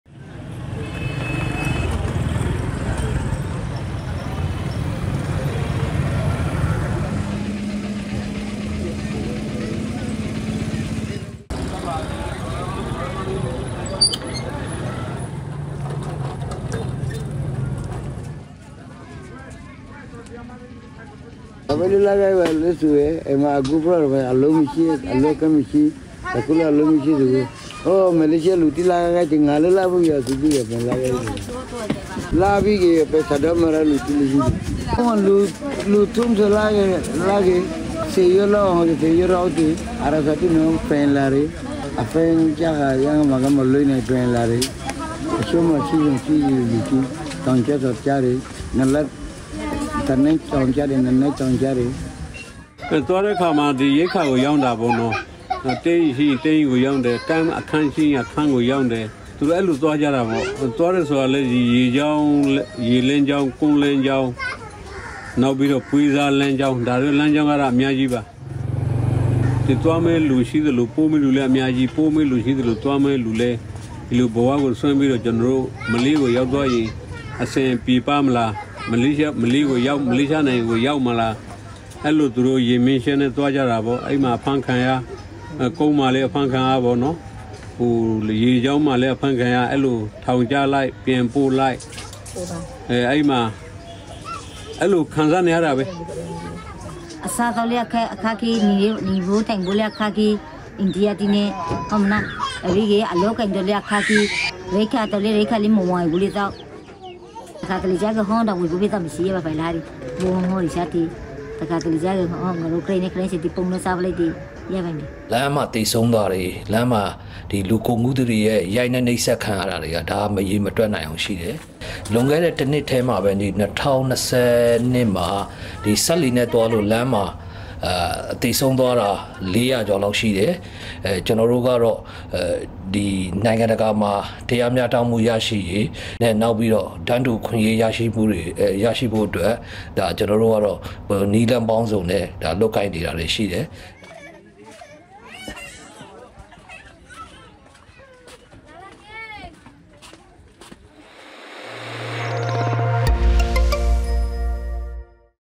“There are no opportunities in the camp,” Rohingya Muslim refugee in Myanmar | Radio Free Asia (RFA)
Rohingya Muslims are fleeing refugee camps in western Myanmar’s Rakhine state due to hardship and repression.